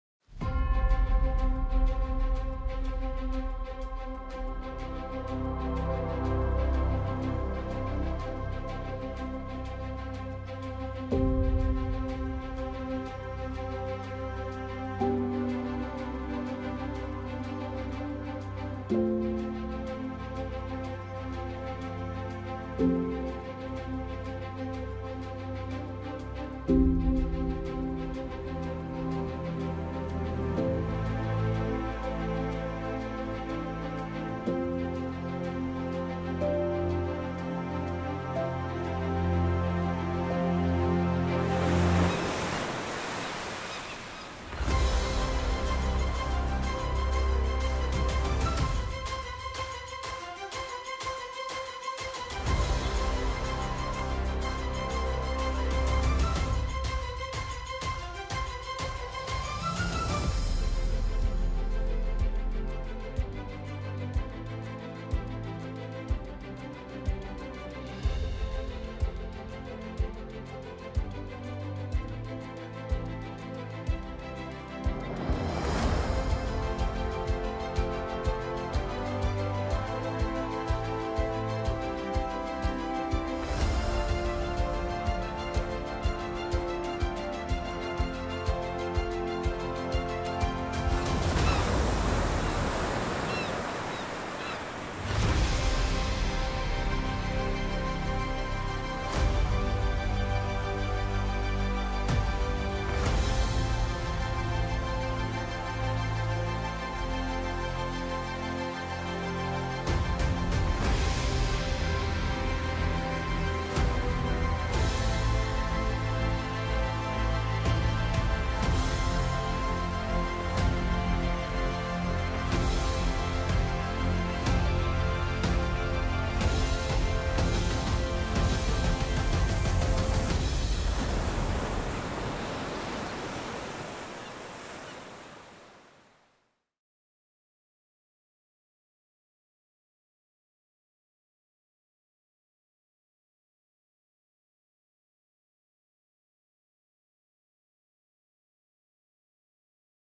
Beautiful Natural Heritage of Korea, Dokdo Island Natural Reserve
October 25 is Dokdo Day, established to express the will to protect and  declare that Dokdo belongs to the Republic of Korea.